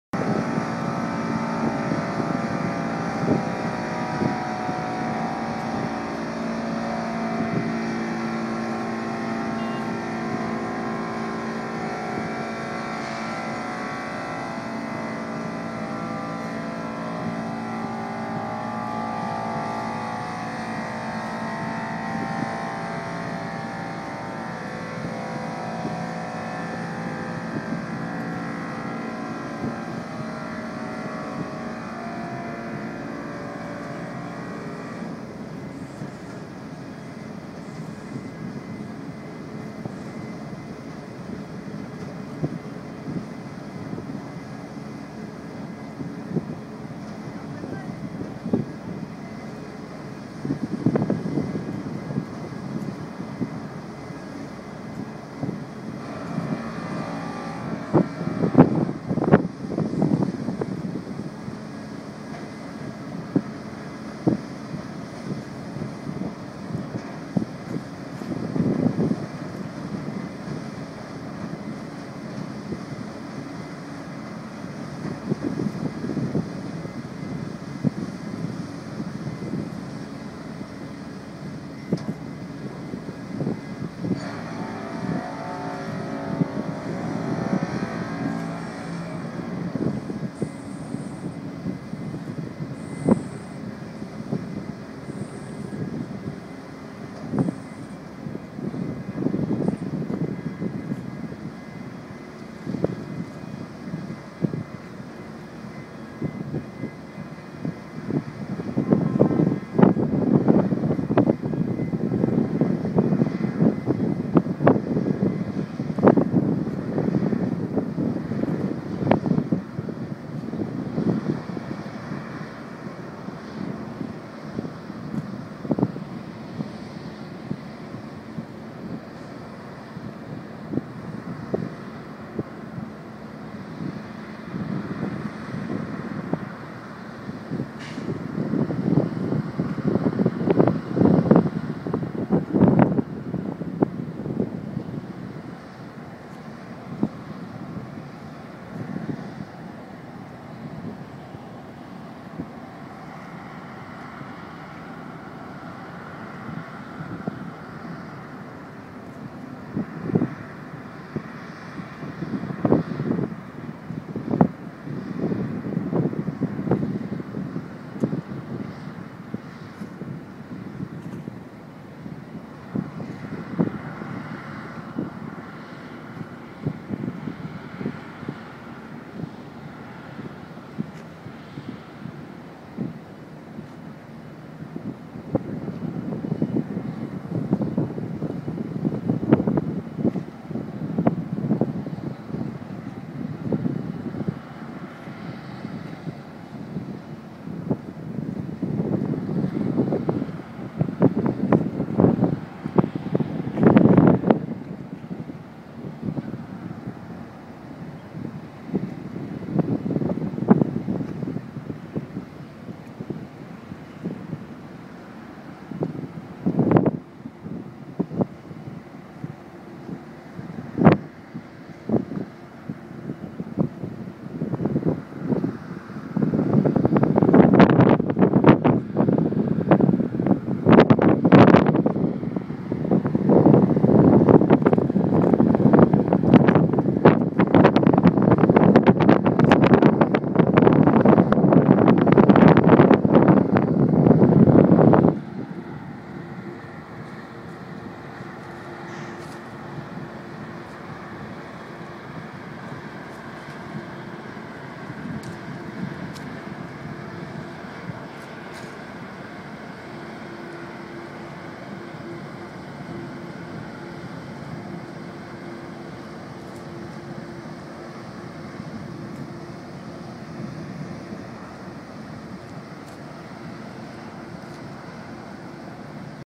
Departure of Highspeed 6 from Piraeus port.
First Departure of Highspeed 6 from Piraeus port to Ios-Santorini. Date 1/6/2012. Time 17:00.